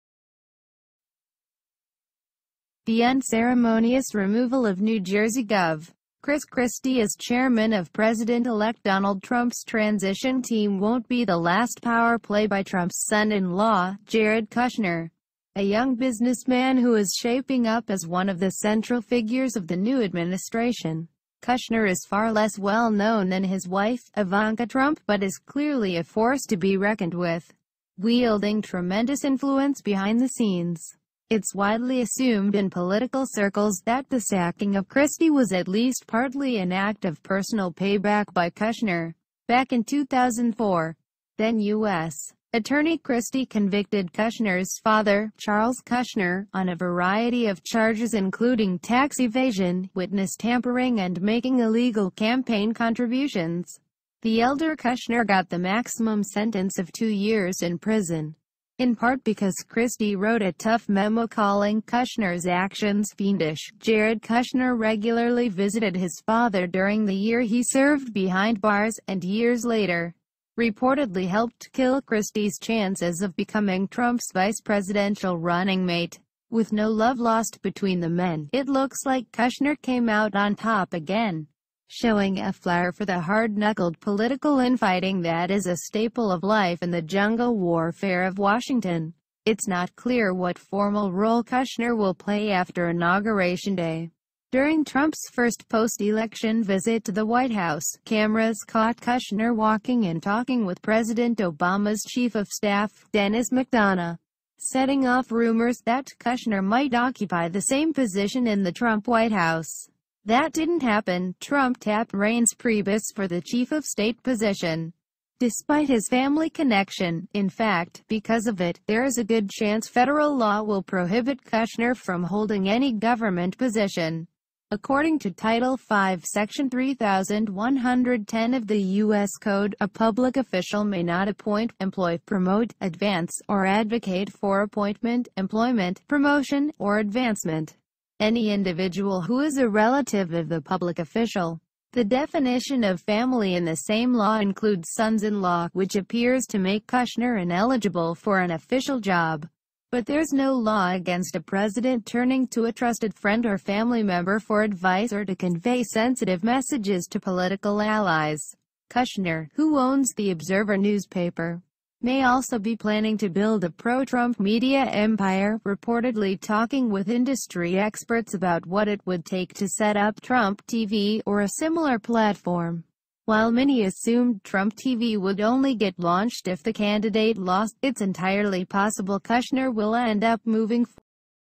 Early Trump-world lesson: Don't cross Jared Kushner
The unceremonious removal of New Jersey Gov. Chris Christie as chairman of President-elect Donald Trump's transition team won't be the last power play by Trump's son-in-law, Jared Kushner, a young businessman who is shaping up as one of the central figures of the new administration.
Kushner is far less well known than his wife, Ivanka Trump, but is clearly a force to be reckoned with, wielding tremendous influence behind the scenes.
It's widely assumed in political circles that the sacking of Christie was at least partly an act of personal payback by Kushner. Back in 2004, then-U.S. Attorney Christie convicted Kushner's father, Charles Kushner, on a variety of charges including tax evasion, witness tampering and making illegal campaign contributions.
The elder Kushner got the maximum sentence of two years in prison, in part because Christie wrote a tough memo calling Kushner's actions "fiendish." Jared Kushner regularly visited his father during the year he served behind bars -- and years later, reportedly helped kill Christie's chances of becoming Trump's vice-presidential running mate.
With no love lost between the men, it looks like Kushner came out on top again, showing a flair for the hard-knuckled political infighting that is a staple of life in the jungle warfare of Washington.
It's not clear what formal role Kushner will play after Inauguration Day. During Trump's first post-election visit to the White House, cameras caught Kushner walking and talking with President Obama's chief of staff, Denis McDonough, setting off rumors that Kushner might occupy the same position in the Trump White House.
That didn't happen: Trump tapped Reince Priebus for the chief of state position. Despite his family connection -- in fact, because of it -- there's a good chance federal law will prohibit Kushner from holding any government position. According to Title 5 Section 3110 of the US Code, "a public official may not appoint, employ, promote, advance, or advocate for appointment, employment, promotion, or advancement ... any individual who is a relative of the public official."
The definition of family in the same law includes sons-in-law, which appears to make Kushner ineligible for an official job. But there's no law against a president turning to a trusted friend or family member for advice or to convey sensitive messages to political allies.
Kushner, who owns the Observer newspaper, may also be planning to build a pro-Trump media empire, reportedly talking with industry experts about what it would take to set up "Trump TV" or a similar platform. While many assumed Trump TV would only get launched if the candidate lost, it's entirely possible Kushner will end up moving forward anyway.
As Christie, among others, has discovered the hard way, it's a mistake to ignore or underestimate one of the youngest and savviest political players on Team Trump.